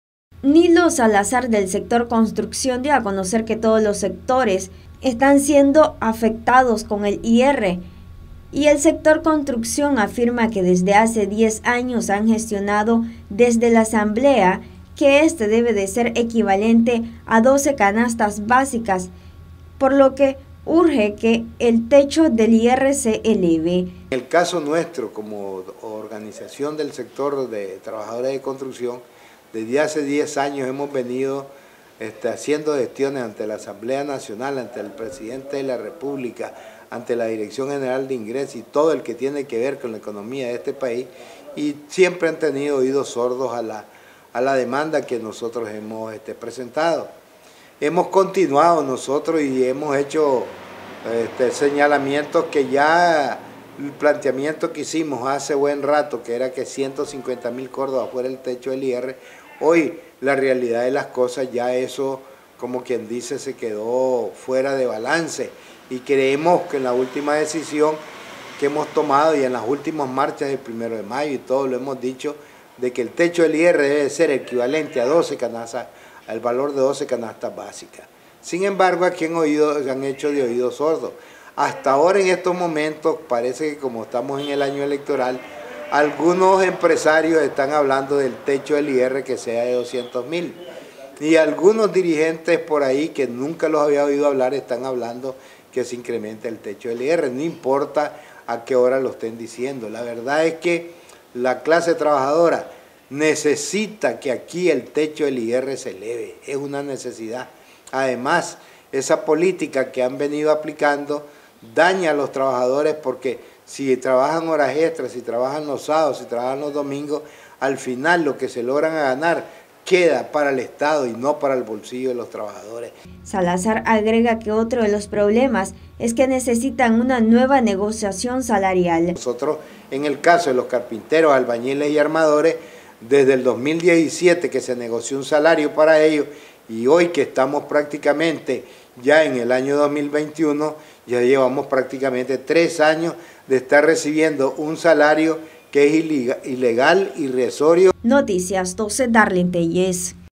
Sector construcción buscará un nuevo salario
NosResistimosAlEmbargo Sector construcción buscará un nuevo acuerdo salarial, dijo el sindicalista Nilo Salazar.